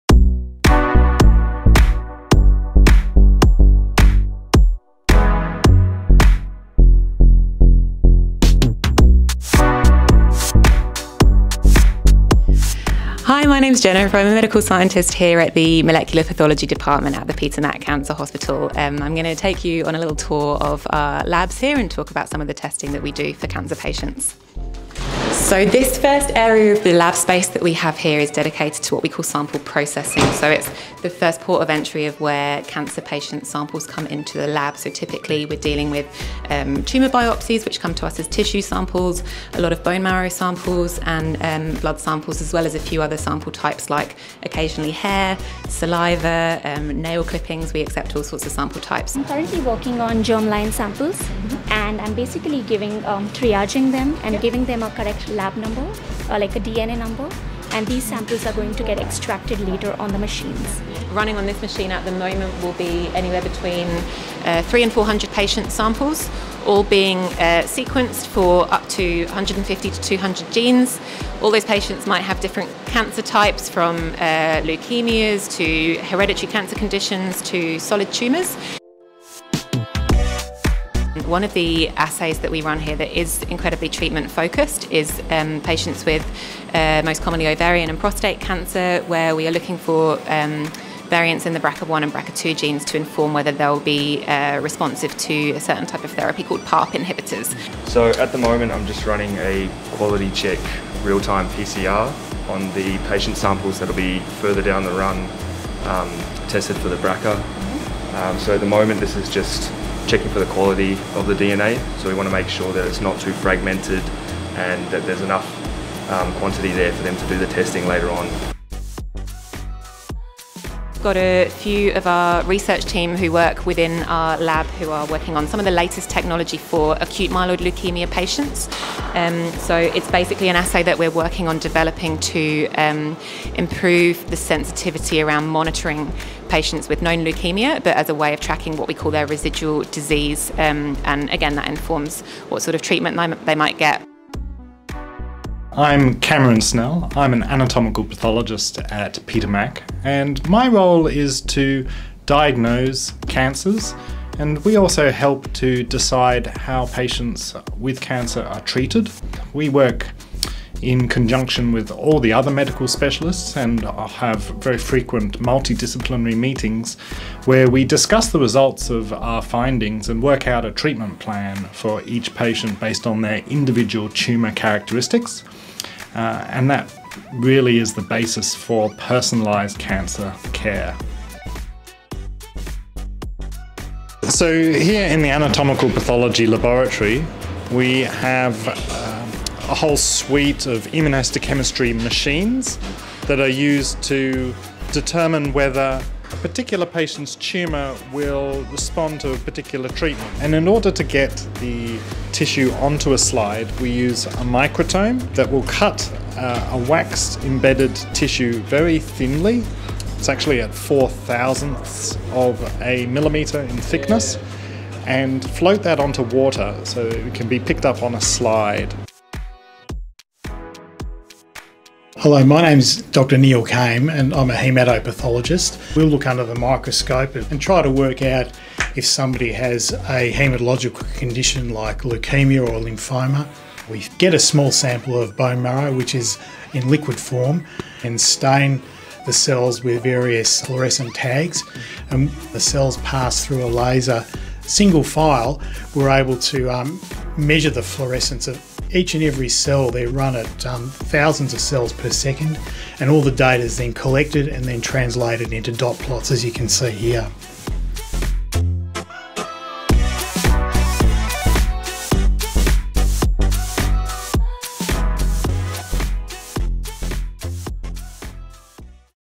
Behind the scenes in Peter Mac Pathology - International Pathology Day 2022
Pathology is a medical specialty about the study and causes of disease and is integral to the diagnosis of every cancer.

Pathologists can also examine changes in tissue, blood and other bodily fluid samples to gain an understanding of the potential for disease development, the cause or severity of disease and/or to monitor the progress of disease, effectiveness of treatment and your general health.

At Peter Mac, the Pathology team uses blood and other tissue samples to help diagnose the tumour type and can also help determine a likely prognosis and inform clinicians about the most appropriate management for a cancer.

Peter Mac’s Pathology team operates a 24-hour on-site service which provides biochemistry, haematology, anatomical pathology and diagnostic molecular pathology.

Patient bloods or specimens collected during any sort of procedure, as an inpatient or in our outpatient pathology clinic - go straight to our on-site pathologists for analysis.